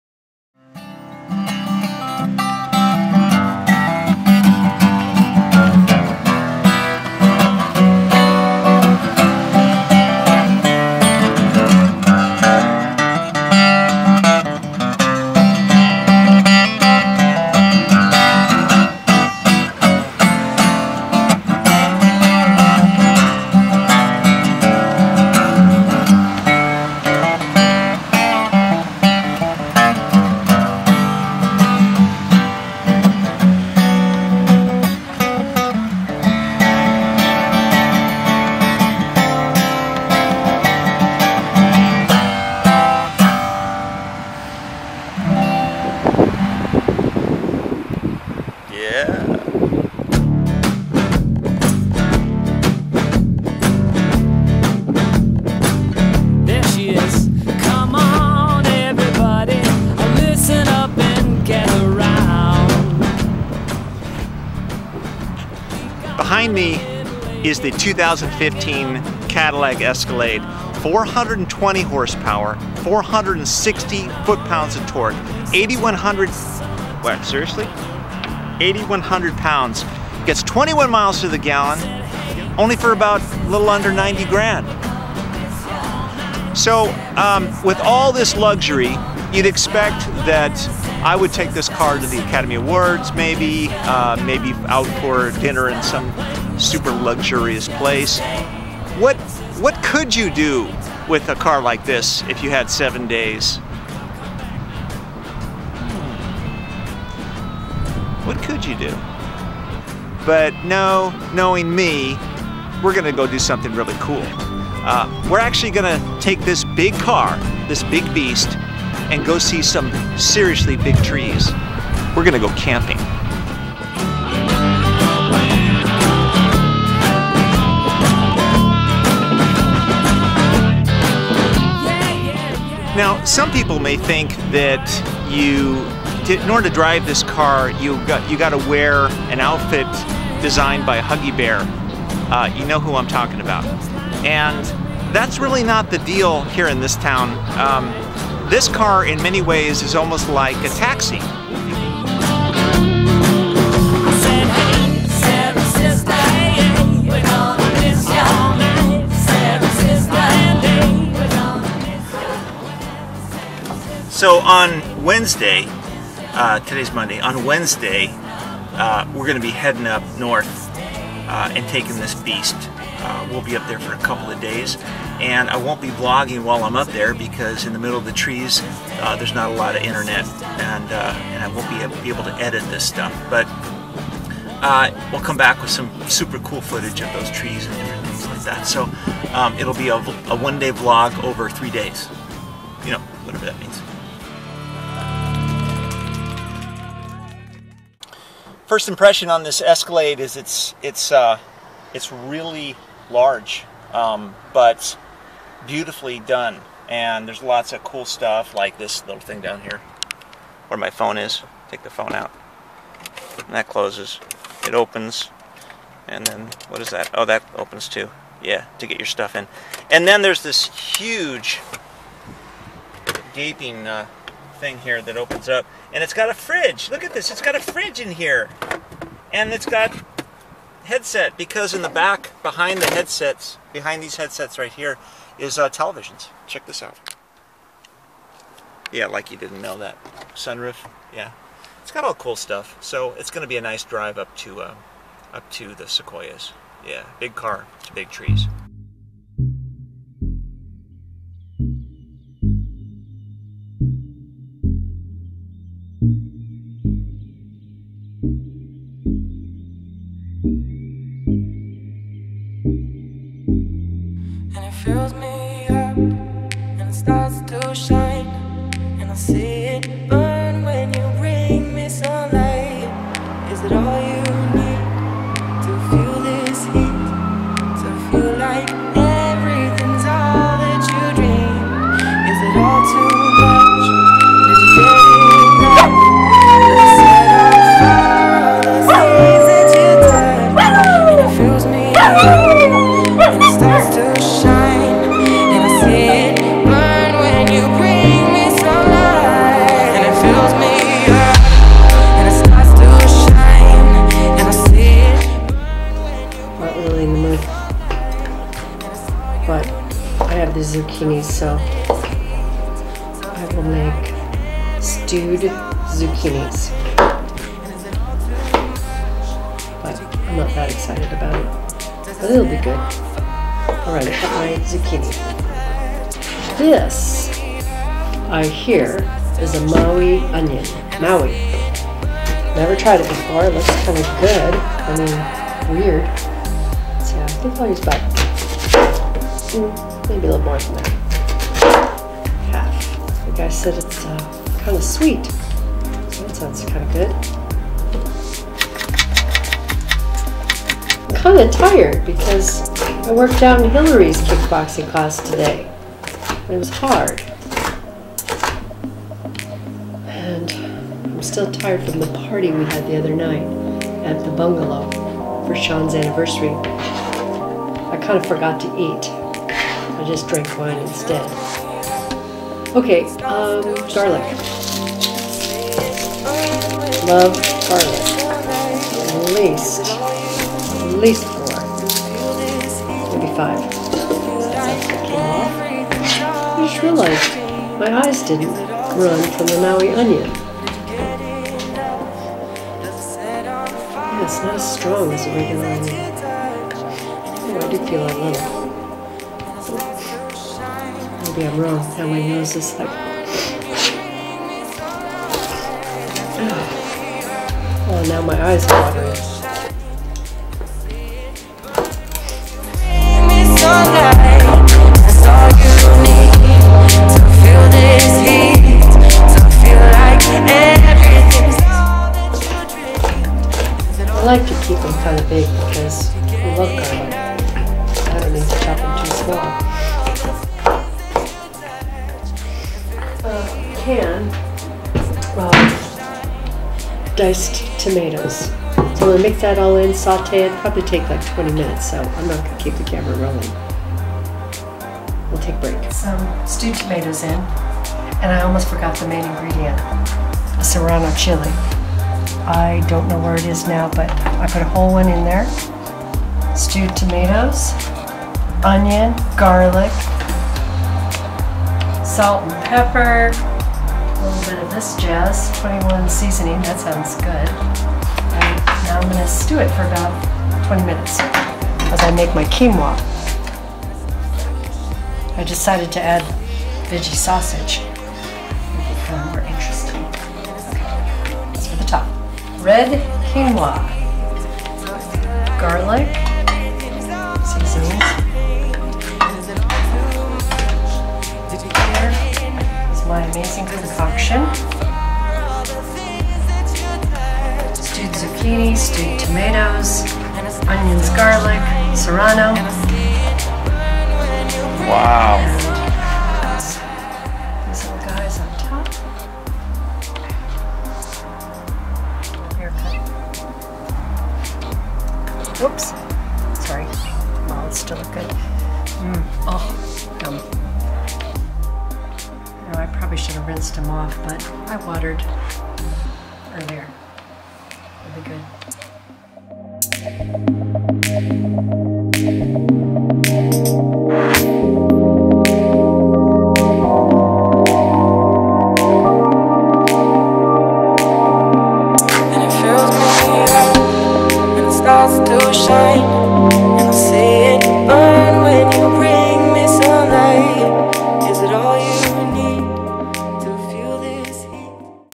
2015 CADILLAC ESCALADE - FIREBALL MALIBU VLOG 14
2015 CADILLAC ESCALADE - FIREBALL MALIBU VLOG 14 - Fireball gets a brand new 2015 Cadillac Escalade, then makes serious plans as Kathie forms a wild spicy concoction.

2015 CADILLAC ESCALADE - FIREBALL MALIBU VLOG 14